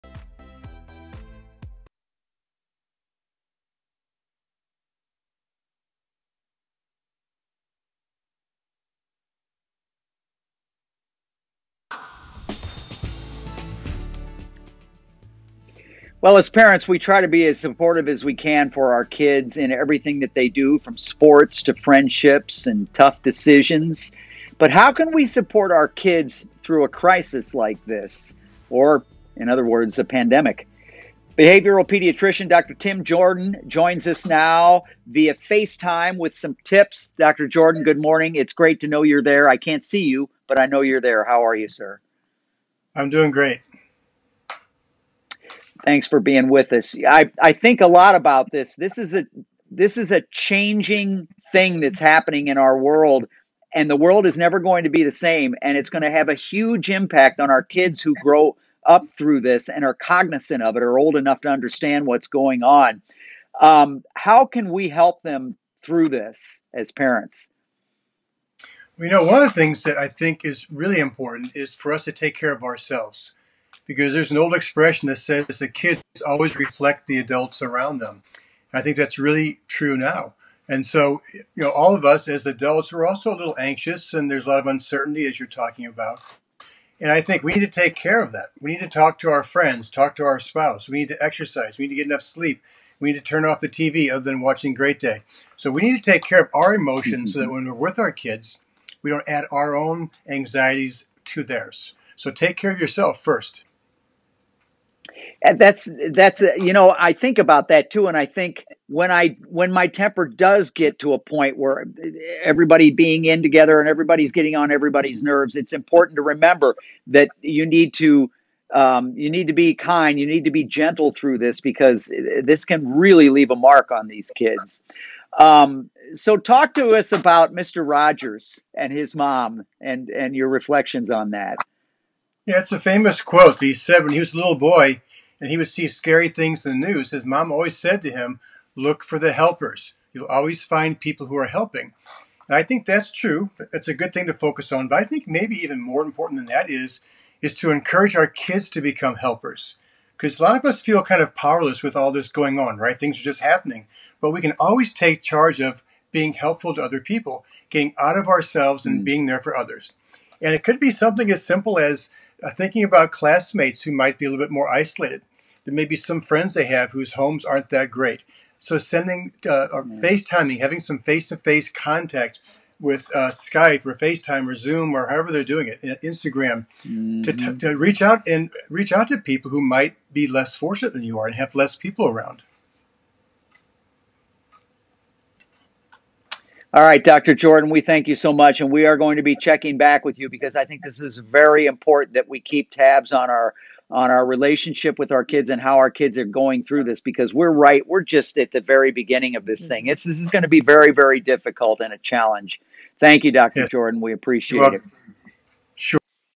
Dr. Tim Jordan: Parenting Through Crisis
Dr. Tim Jordan offers some parenting tips that will help you & your children get through the current pandemic.